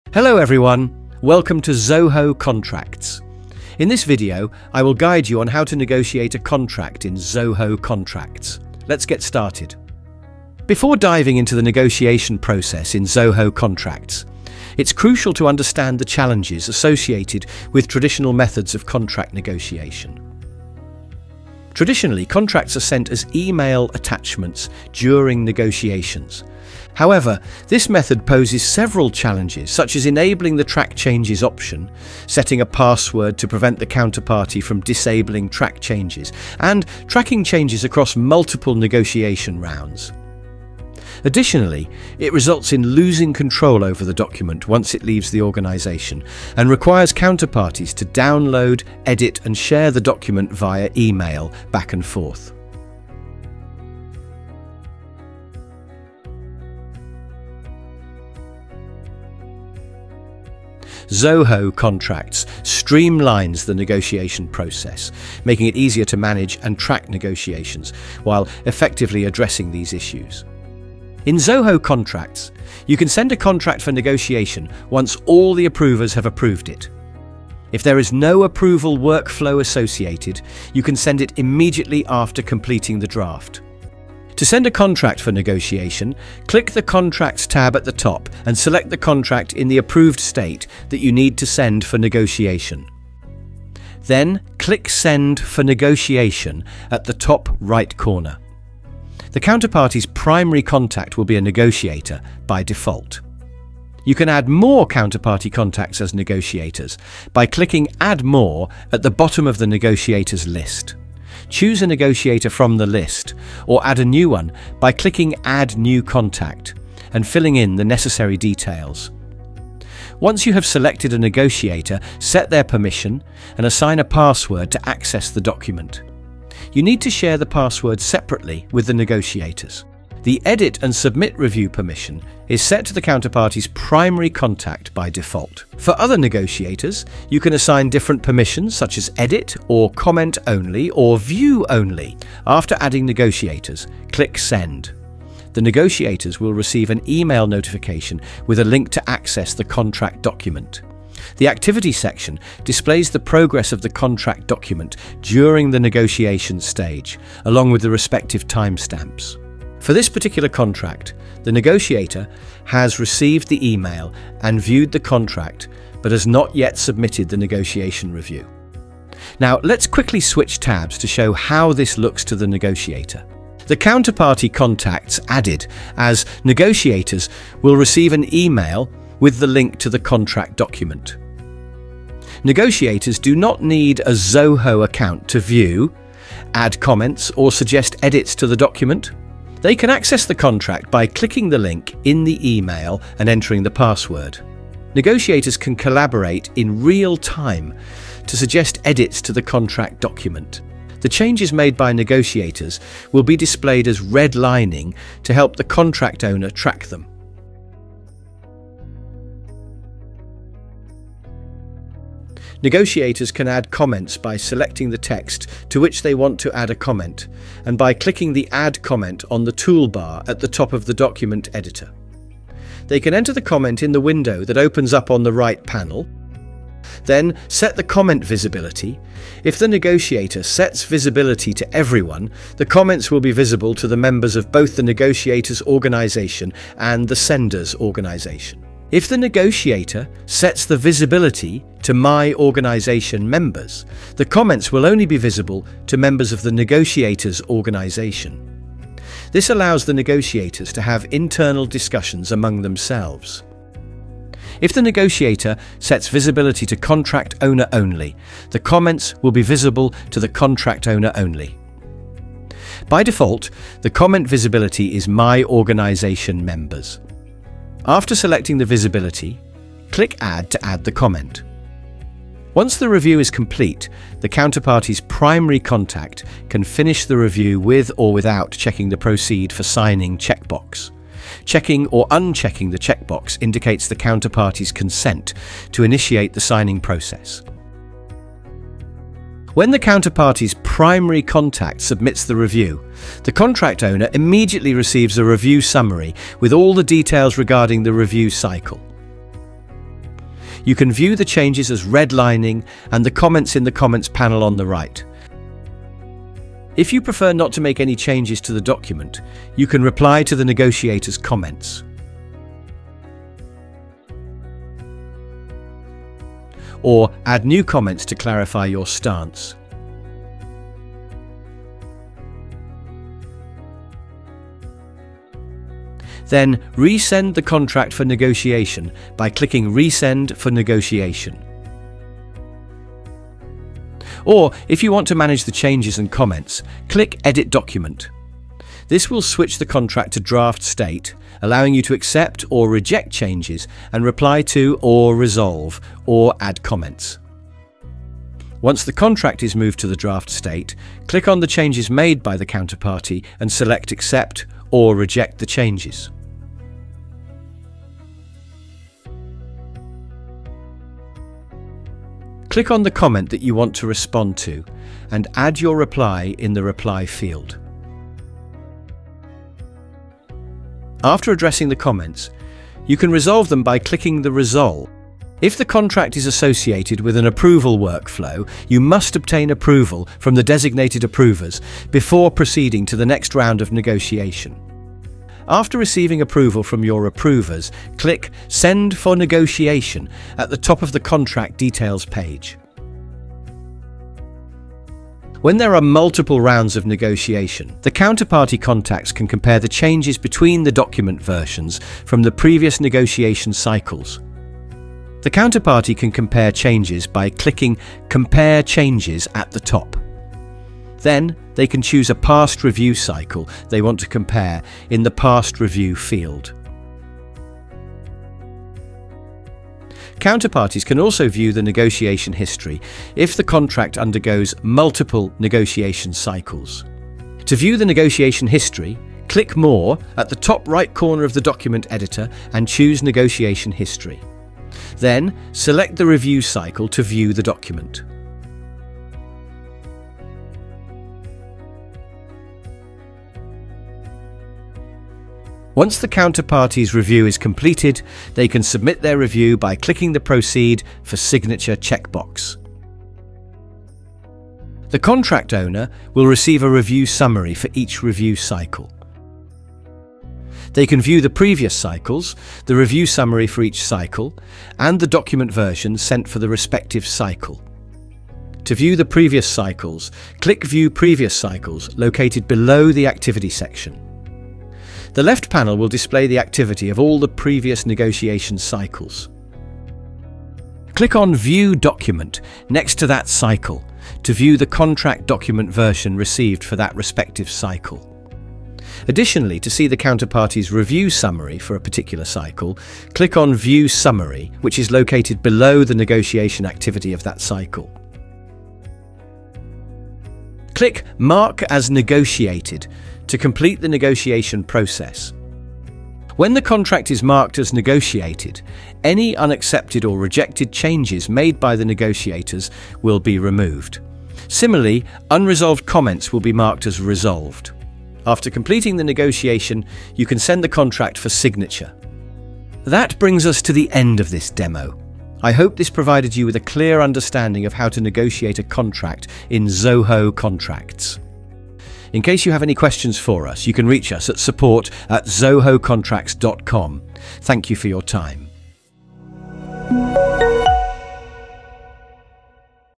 Zoho Contracts | How to Negotiate a Contract | Tutorial
Learn to negotiate a contract with Zoho Contracts through this video. It covers the entire negotiation process, from sending and tracking contracts to managing negotiations efficiently.